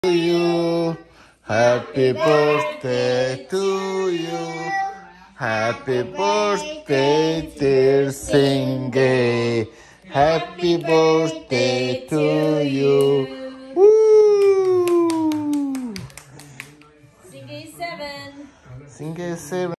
Sengee
Our little guy turn 7 years old. Jetsun lead sing and Tengher just happy to be clapping